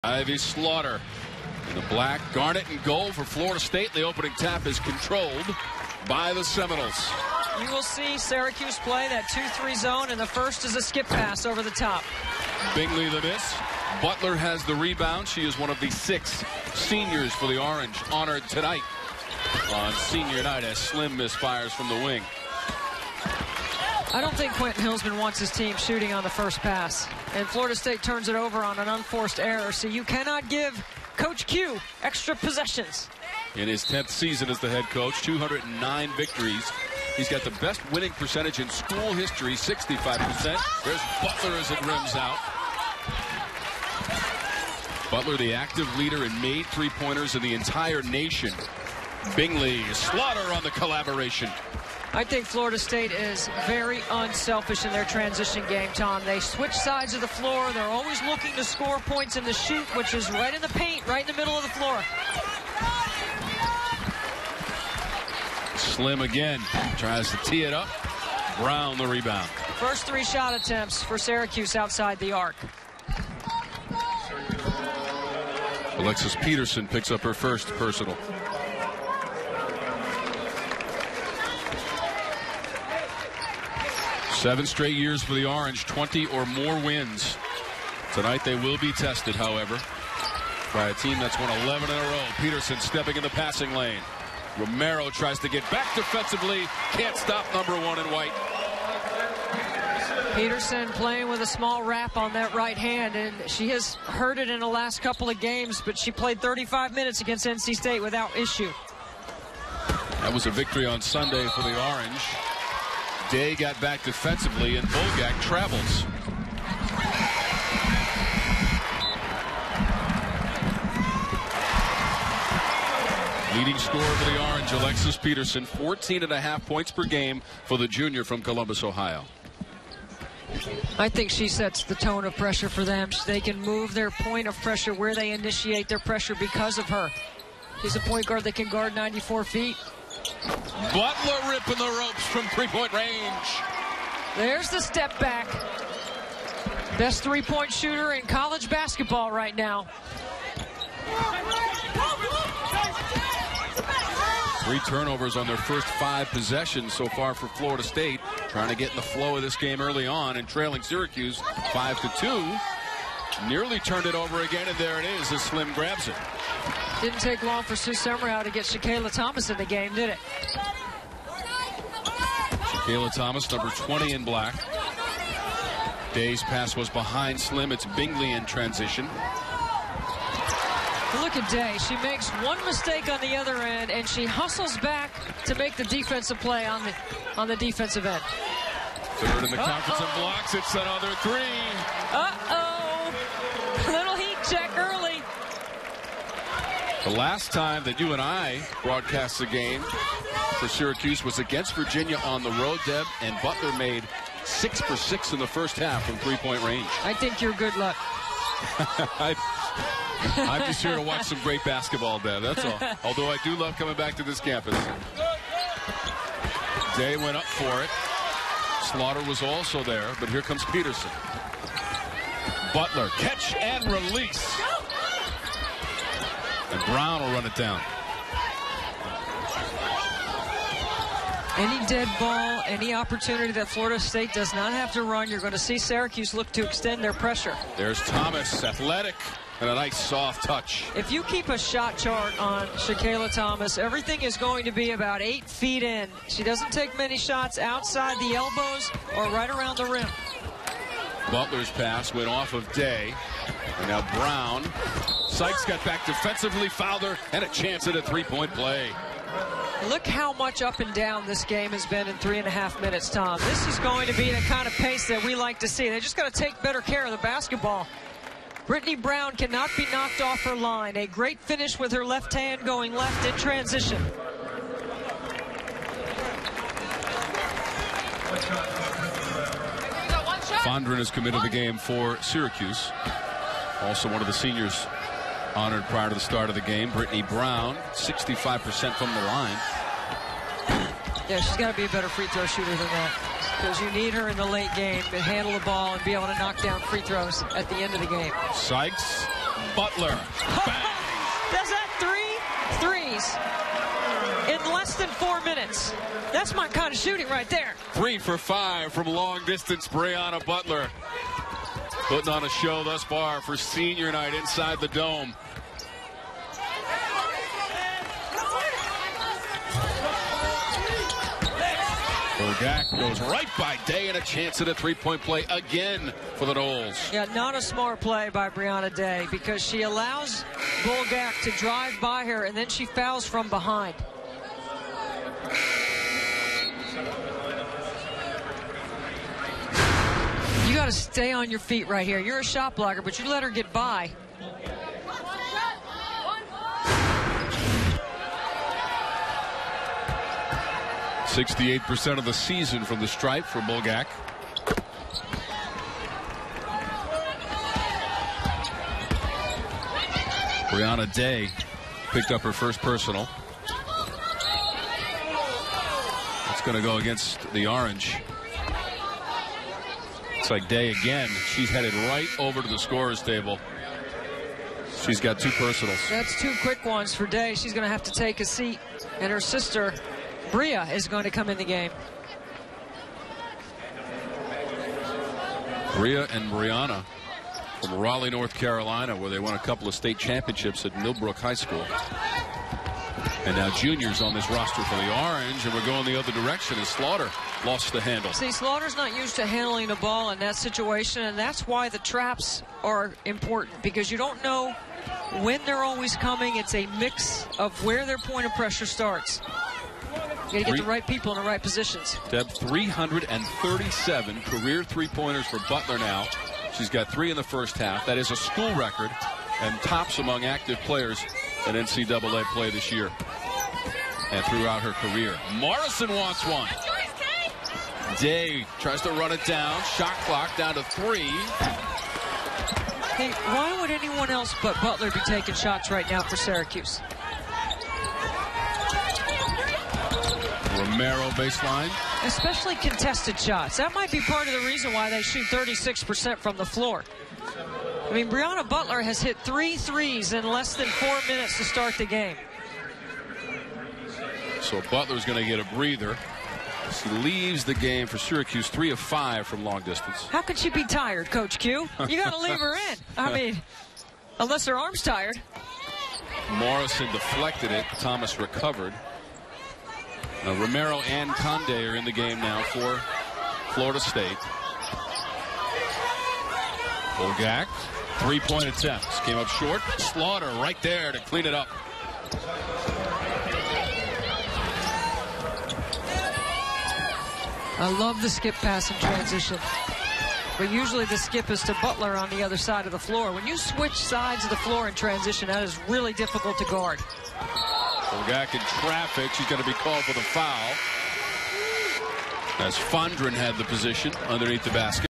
2/14 Syracuse 83 - 73 Florida State (NCAA 2015/2016 season; ACC conference)
Syracuse 83 - 73 Florida State (NCAA 2015/2016 season; ACC conference). Game played in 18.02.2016.

FSU: #2 black: Adut Bulgak: 22 MIN; 23 PCT (9/12 2PCT; 5/7 FT); 9 REB; 1 STL; 20 EFF